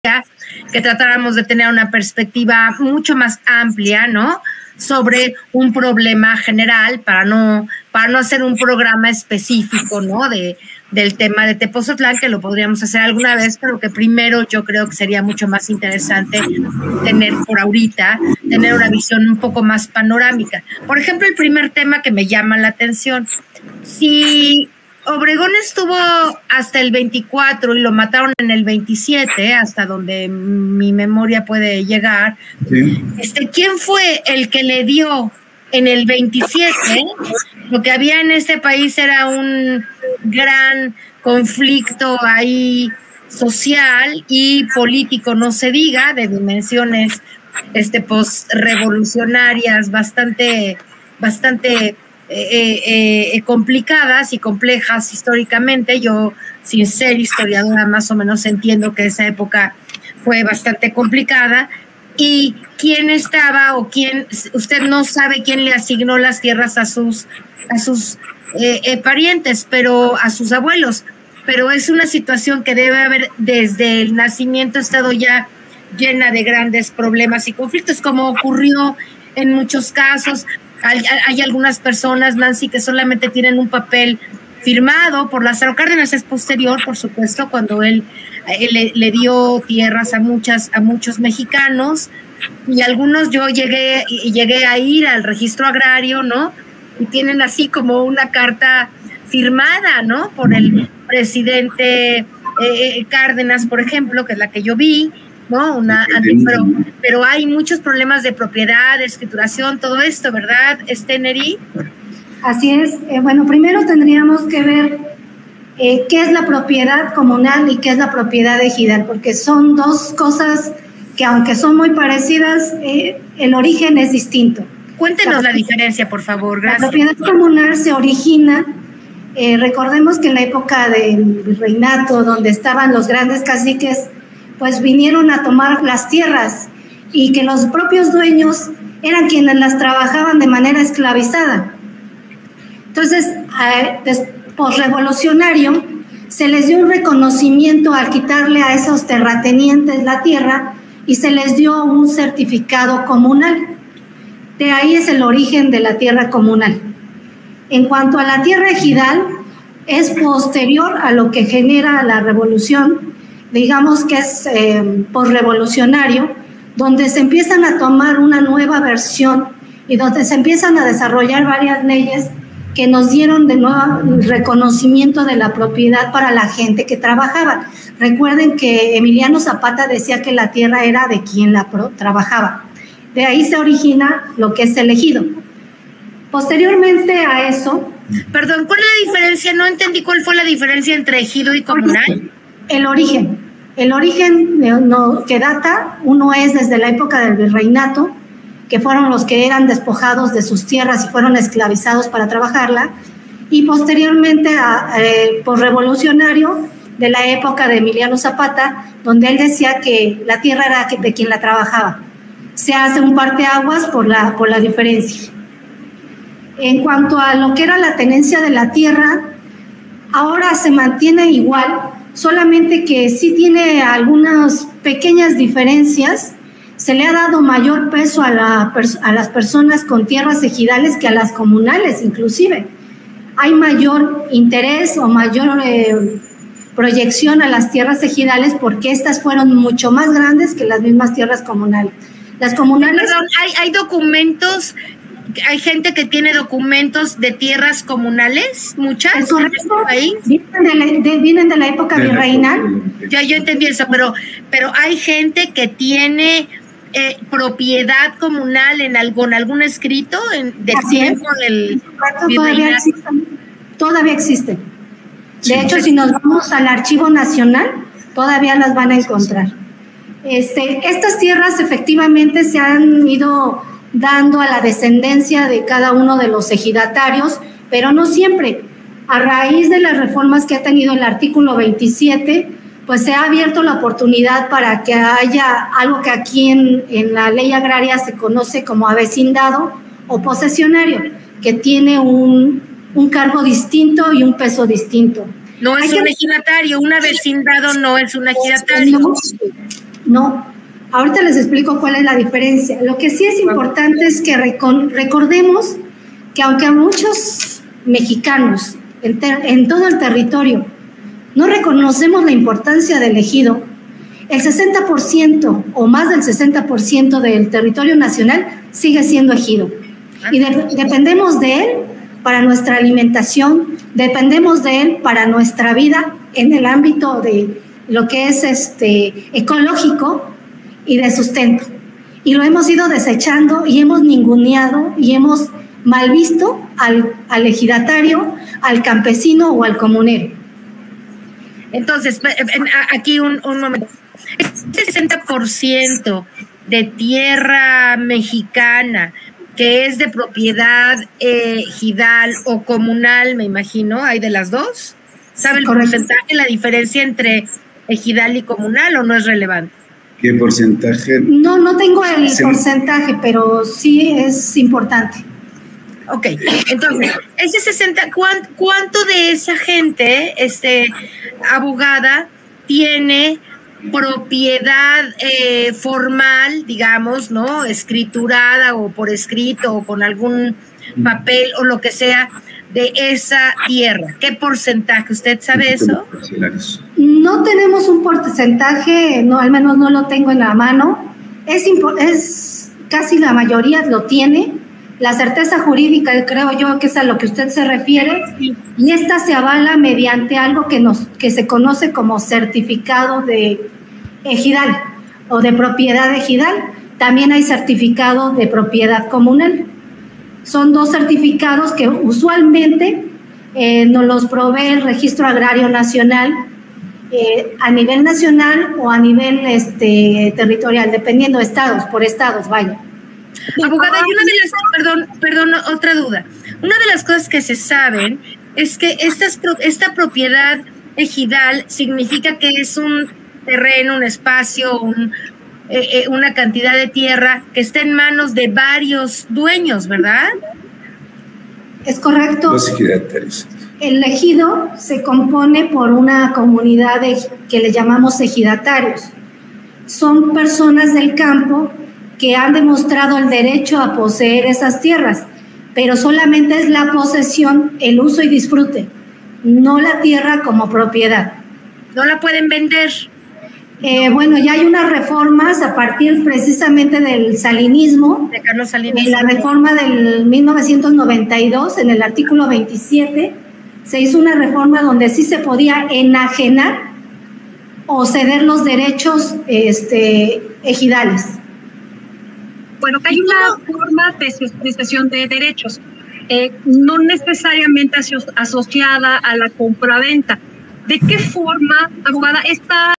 EJIDATARIOS SOLO PUEDEN UTILIZAR LAS TIERRAS PERO NO SON PROPIETARIOS DE ELLAS: MARTHA NERI HERRERA
La licenciada Martha Neri Herrera explicó que los ejidatarios solo pueden utilizar y disfrutar las tierras pero no son propietarios de las mismas